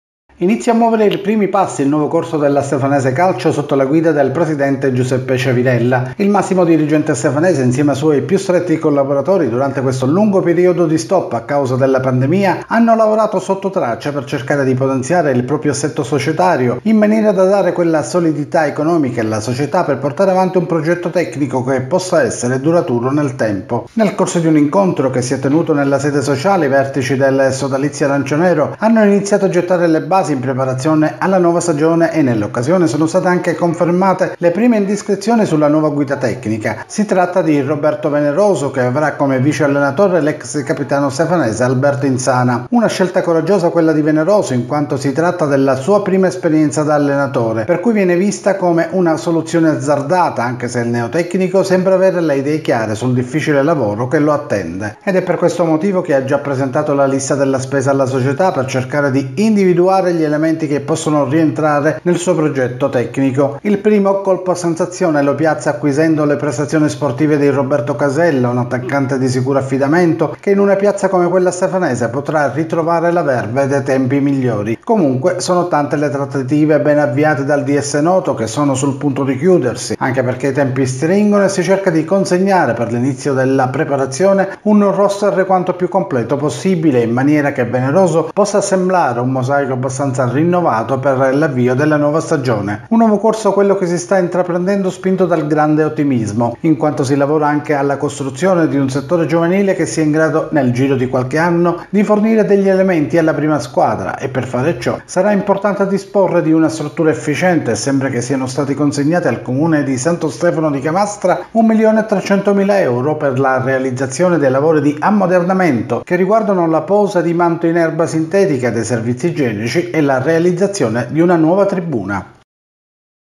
Promozione gir.B, la Stefanese esonera Roberto Veneroso
Il complicato inizio nel campionato di promozione, con tre sconfitte in altrettante gare, ha spinto la dirigenza della Stefanese a sollevare dall'incarico di allenatore Roberto Veneroso. Un modo per dare una scossa all'ambiente nero-arancio